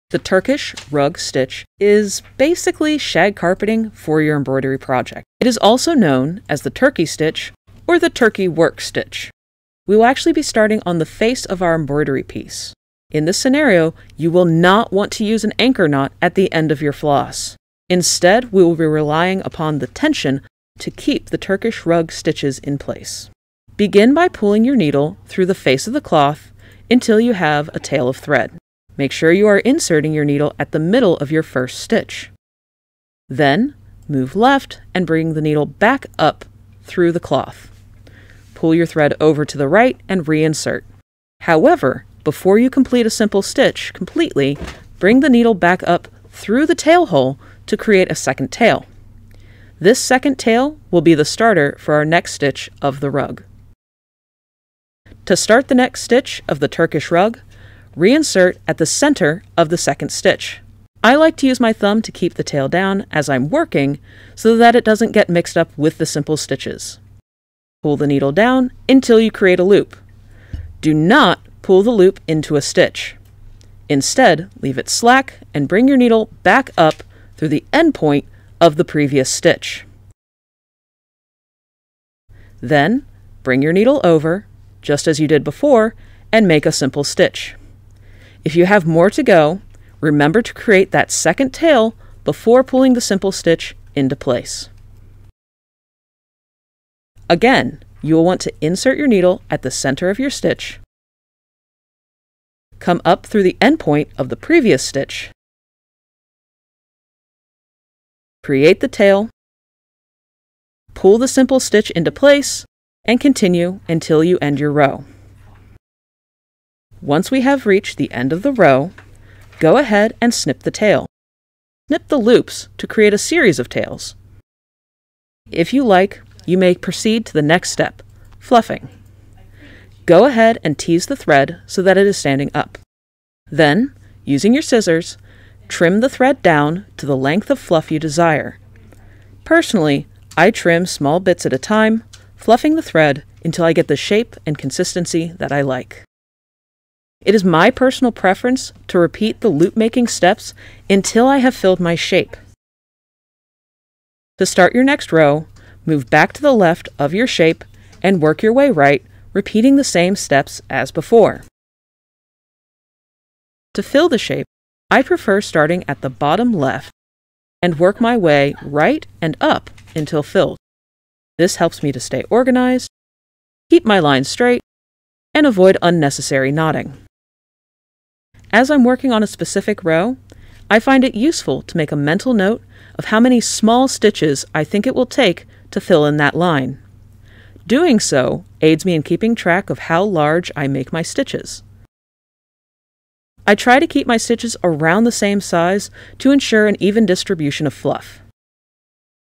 20 Turkish Rug Stitch
A thread intensive tool that creates fuzzy patches for an embroidery piece. Good for fur, grass, flames, or other items of that nature.